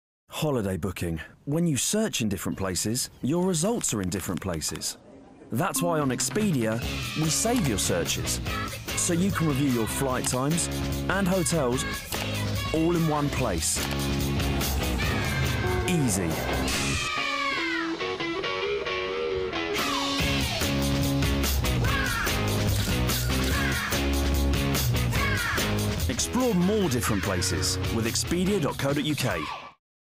Explore more different places with Expedia!
Holiday booking... When you search in different places, your results are in different places. That’s why on Expedia, we save your searches.
So you can review your flight times and hotels all in one place.
Easy.